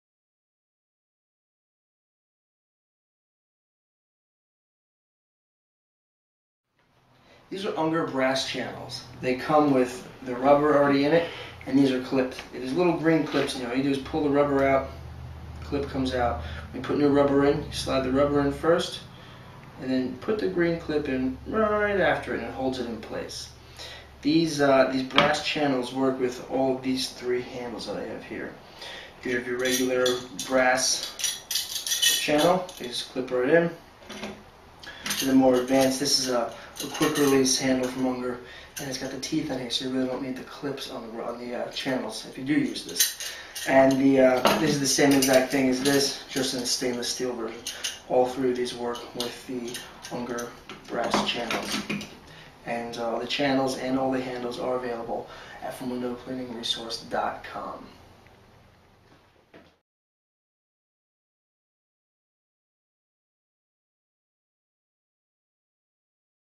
Unger Brass Channels Handles
The Unger Brass Squeegee Channels come complete with Unger ErgoTec Soft Rubber. These clipped Channels are best used with the Unger Golden Pro handle.

Have any questions about this product, another product, or just want to place an order, give us a call at or go